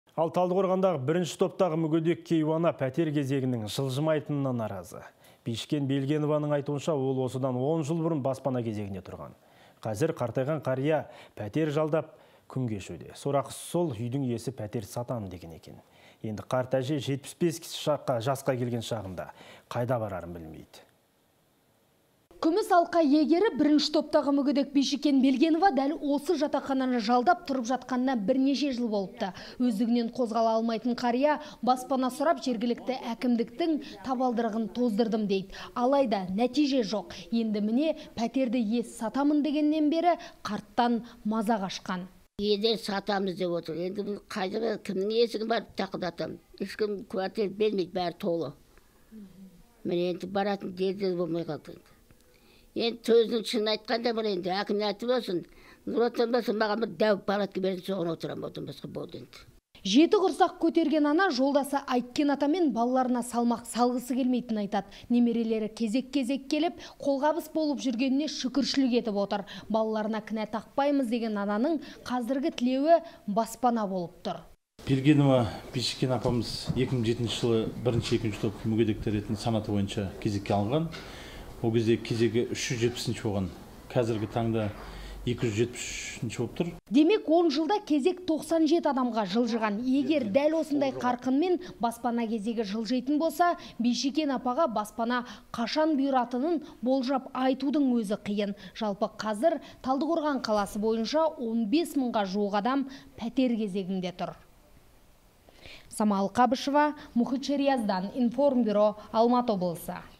Талдықорғандағы бірінші топтағы мүгедек кейуана пәтер кезегінің жылжымайтынына наразы
Талдықорғандағы бірінші топтағы мүгедек кейуана пәтер кезегінің жылжымайтынына наразы. Бишікен Белгенованың айтуынша, ол осыдан он жыл бұрын баспана кезегіне тұрған. Қазір қария пәтер жалдап, күн кешуде. Сорақысы сол, үйдің иесі пәтерді сатамын деген екен. Енді қарт әже 75 жасқа  келген шағында қайда барарын білмейді.